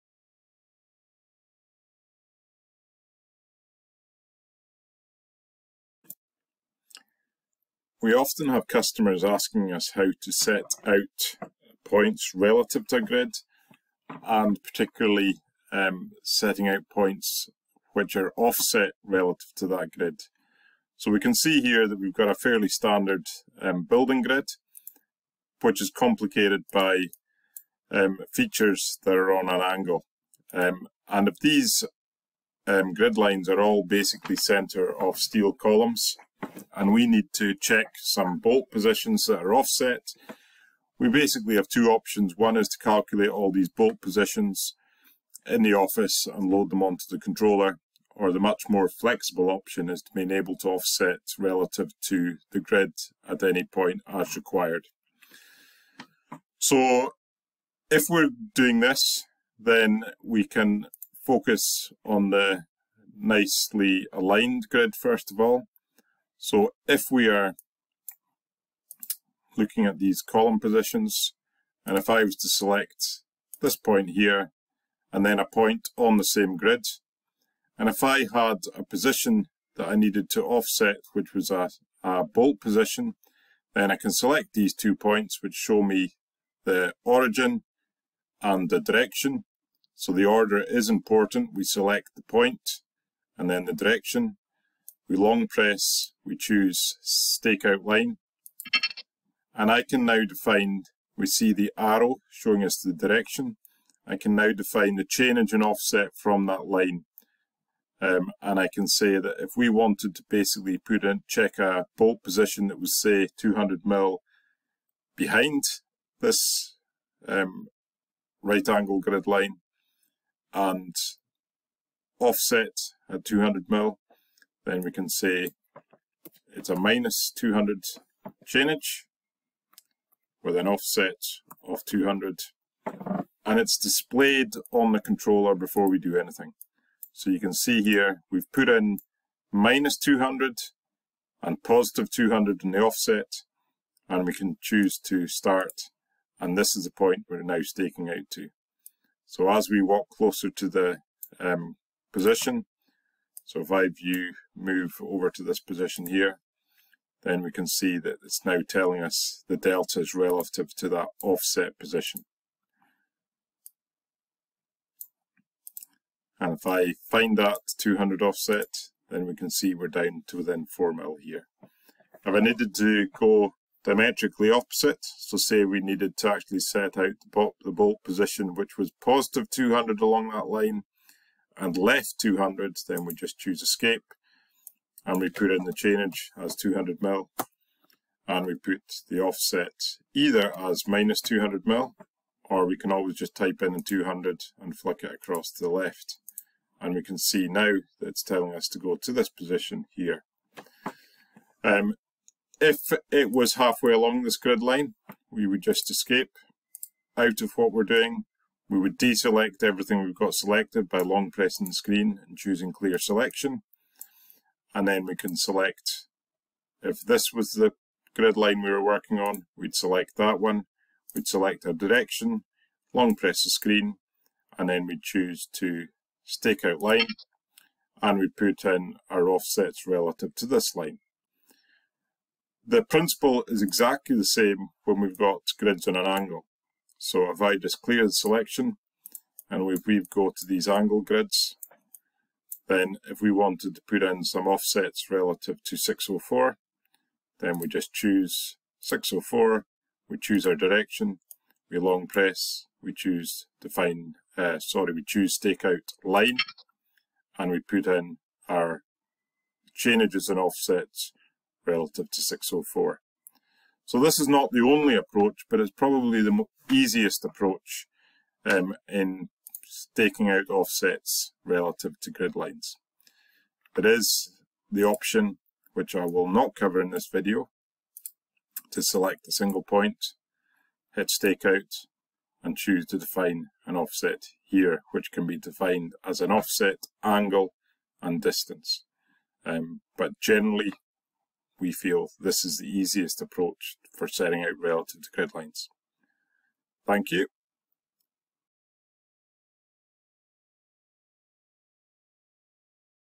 Staking out Relative to Gridlines
This video guides Trimble Access users on how to set out points relative to gridlines using the Reference line command. This is a standard Feature in all versions of Trimble Access